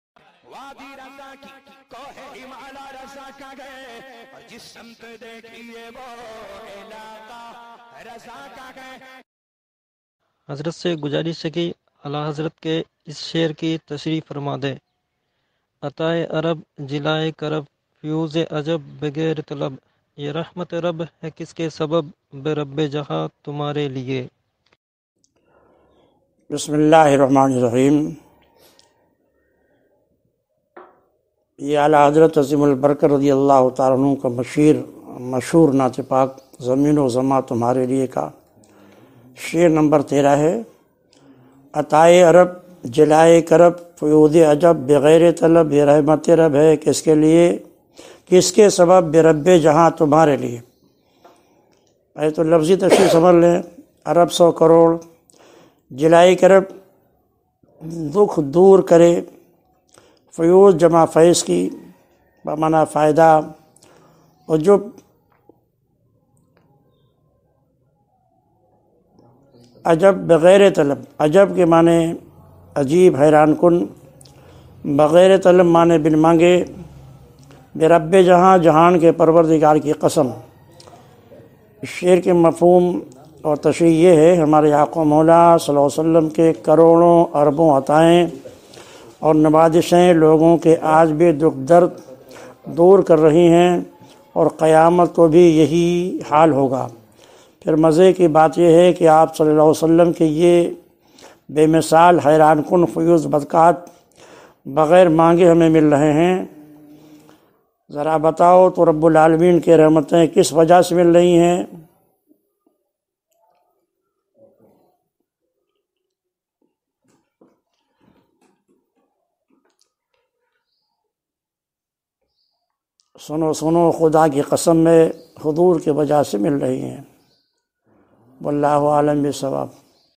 Aala Hazrat ke is sher ki Tashreeh kya hai❓
Aljawab Ustazul Fuqha, Faqih e Asr, Mufti e Azam Allahabad Hazrate Allama wa Maulana Mufti Shafiq Ahmad Sharifi Sahab (Markazi Qazi e Shaher Allahabad)